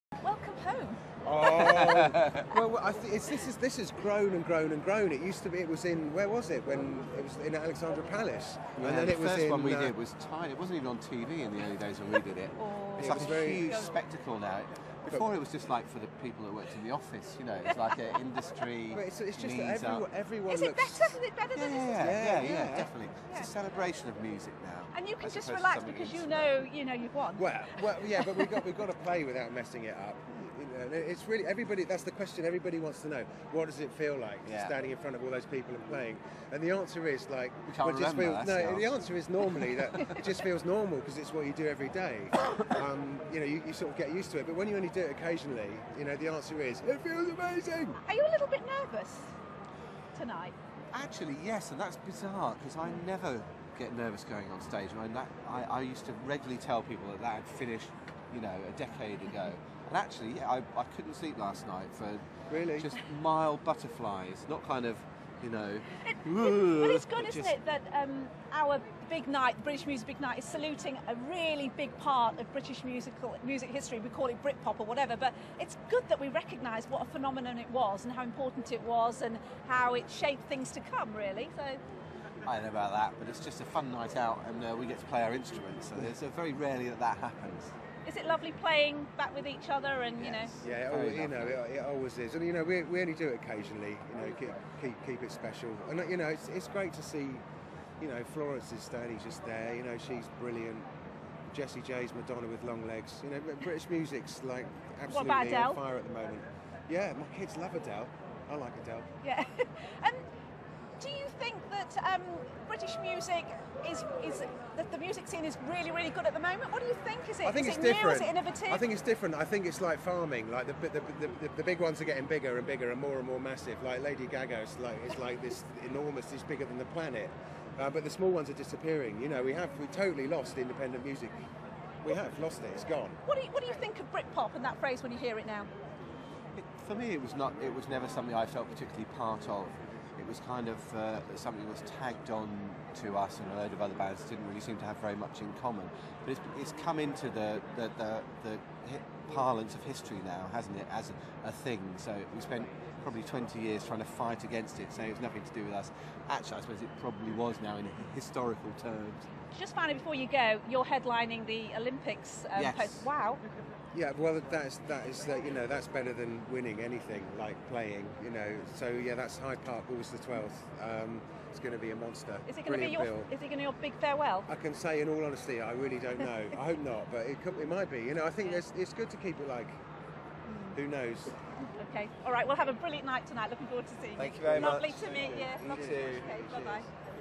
Alex James & Dave Rowntree @ 2012 Brit Awards
Alex & Dave being interviewed at the 2012 Brit Awards discussing about the days of Britpop, 2012 Olympics and thoughts on the New Artists nominated.